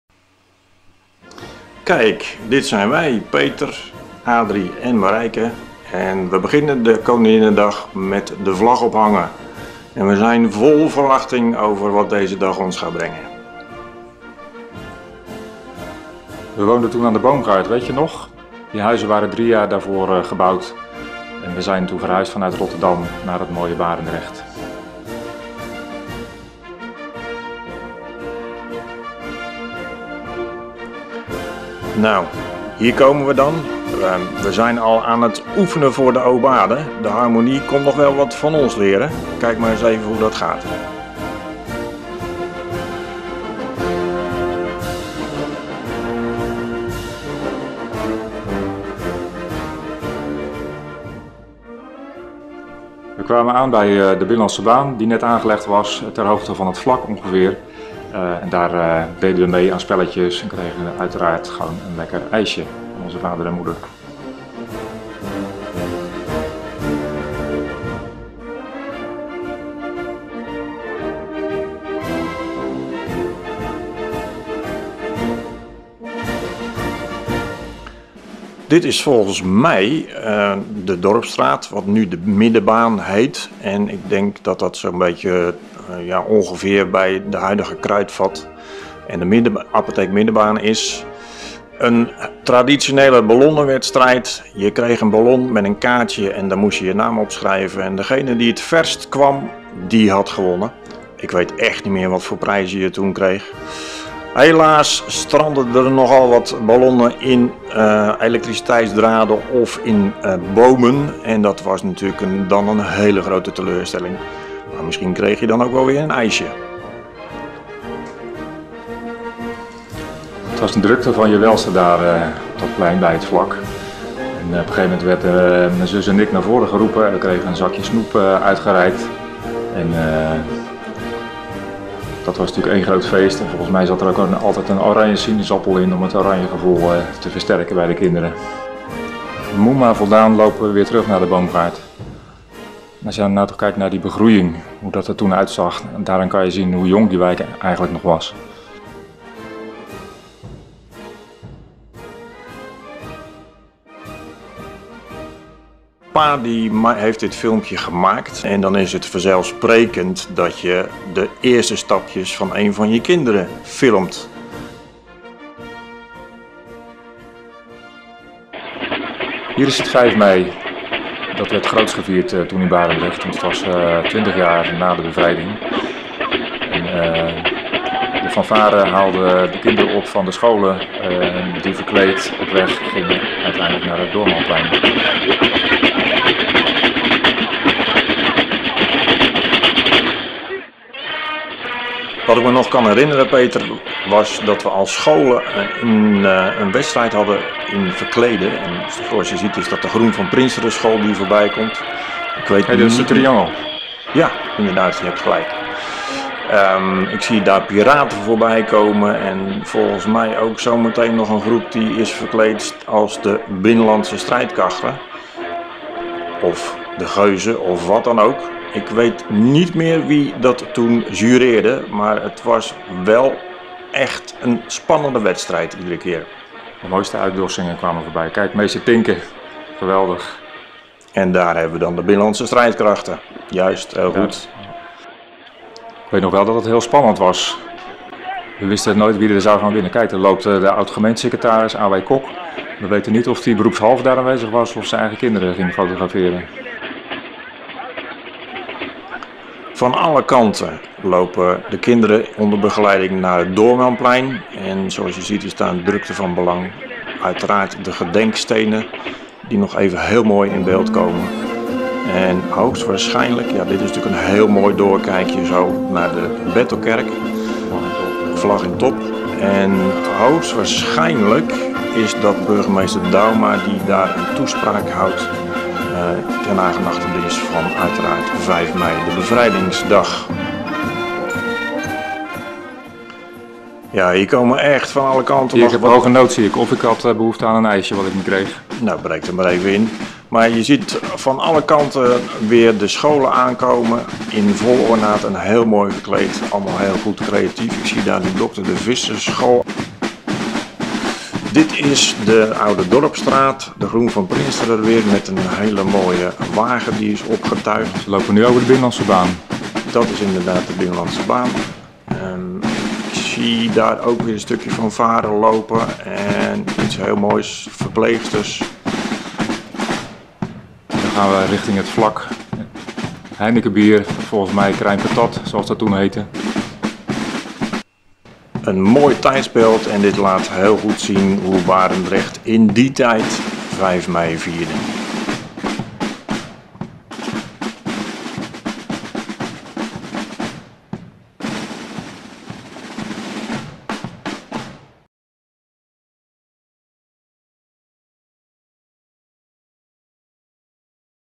Koninginnedag en Bevrijdingsfeest 1965
Twintig jaar na de oorlog pakte Barendrecht uit met een groot Bevrijdingsfeest zonder dat dit ten koste ging van Koninginnedag . Verschillende amateurfilmers hebben die feestdagen toen opgenomen. Tom Oosterom die toen aan de Boomgaard woonde, deed dat ook en maakte mooie beelden van de feesten. Een terugkijkfilm uit de collectie van